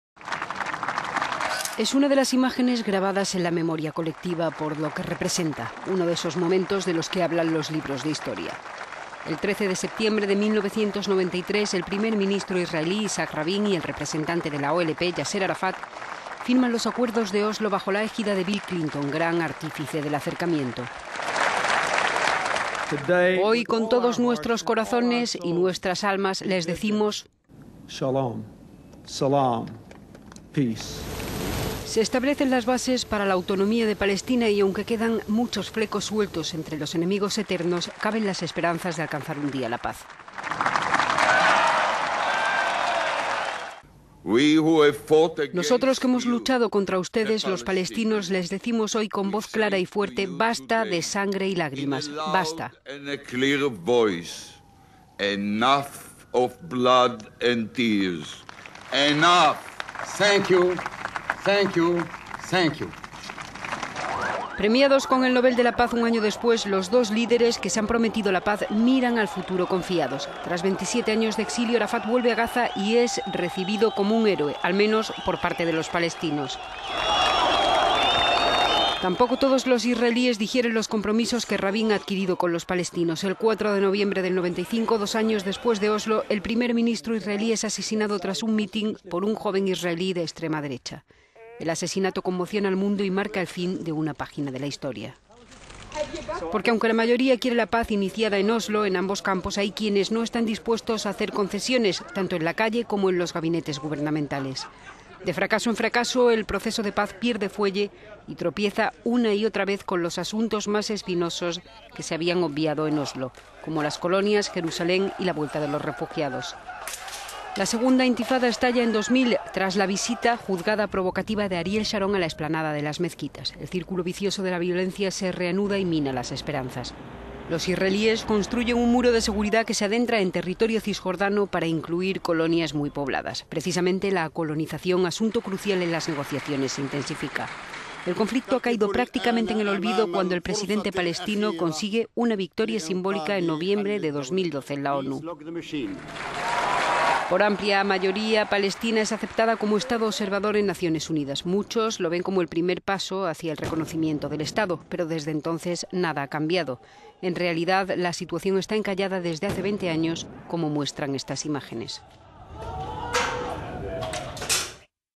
Oslo: 20 años de sueños rotos
Es una de las imágenes grabadas en la memoria colectiva por lo que representa: uno de esos...

Es una de las imágenes grabadas en la memoria colectiva por lo que representa: uno de esos momentos de los que hablan los libros de historia. El 13 septiembre de 1993, el primer ministro israelí, Isaac Rabin, y el representante de las OLP, Yaser Arafat, firman los Acuerdos de Oslo bajo la égida de Bill Clinton, gran artífice del acercamiento.

Bill Clinton. Expresidente de EE.UU:
" Con todos nuestros corazones y nuestras almas les decimos: shalom, salaam, peace."

Se establecen las bases para la autonomía de Palestina  y aunque quedan muchos flecos sueltos entre los enemigos eternos, caben las esperanzas de alcanzar un día la paz.

Isaac Rabin. Exprimer ministro israelí:
"Nosotros que hemos luchado contra ustedes, los palestinos, les decimos hoy con voz clara y fuerte: basta de sangre y lagrimas. Basta. "

Premiados con el Nobel de la Paz un año después, los dos líderes que se han prometido la paz miran al futuro confiados. Tras 27 años de exilio, Arafat vuelve a Gaza y es recibido como un héroe. Al menos por parte de los palestinos. Tampoco todos los israelíes digieren los compromisos que Rabin ha adquirido con los palestinos.  El 4 de noviembre del 95, dos años después de Oslo, el primer ministro israelí es asesinado tras un mítin por un joven israelí de extrema derecha. El asesinato conmociona al mundo y marca el fin de una página de la historia.

Porque aunque la mayoría quiere la paz iniciada en Oslo, en ambos campos hay quienes no están dispuestos a hacer concesiones, tanto en la calle como en los gabinetes gubernamentales. De fracaso en fracaso, el proceso de paz pierde fuelle y  tropieza una y otra vez con los asuntos más espinosos que se habían obviado en Oslo:  como las colonias, Jerusalén y la vuelta de los refugiados palestinos.

Se instala un clima de tensión que alcanza su punto culminante en 2000, tras la visita juzgada provocativa de Ariel Sharon a la explanada de las mezquitas. Estalla la segunda Intifada.

El círculo vicioso de la violencia se reanuda y mina las esperanzas. Los israelíes construyen un muro que de seguridad que se adentra en territorio cisjordano para incluir colonias muy pobladas. Precisamente la colonización, asunto crucial en las negociaciones se intensifica.

El conflicto ha caído prácticamente en el olvido cuando el presidente palestino consigue una victoria simbólica en noviembre de 2012 en la ONU. Por amplia mayoría, Palestina es aceptada como Estado observador en Naciones Unidas. Muchos lo ven como el primer paso hacia el reconocimiento del Estado...